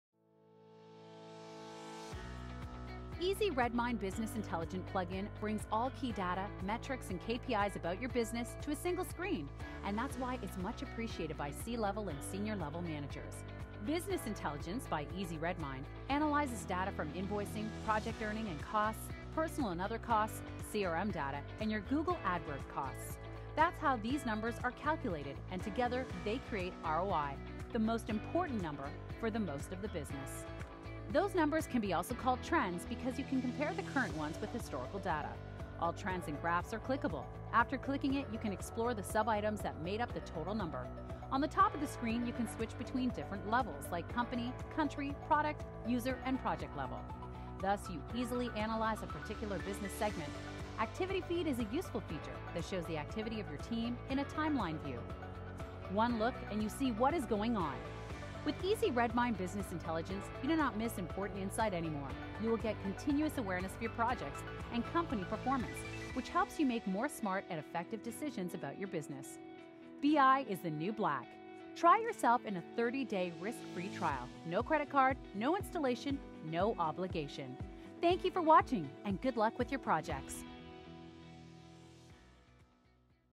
Redmine Business Intelligence (new plugin) – Easy Redmine 10
BI brings you important insights and overview about your projects and company performance. All key data, metrics and KPIs about your projects & business on a single well-arranged screen.

The most important ROI in particular business segment can be viewed by product / project / country and it's calculated based on:

– incomes & revenues (invoicing)
– project earnings
– project costs
– personal costs (spent time X hourly rates)
– other costs
– CRM data
– campaign cost (Google Adwords connection)